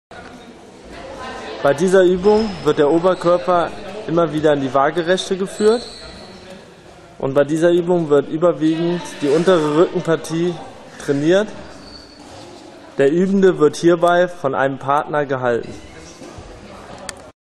Rückenkräftigung - Oberkörperheben (2)
Es wird gezeigt, wie die konditionelle Komponente Kraft in der schulischen Praxis möglichst effektiv und regelmäßig geschult werden kann.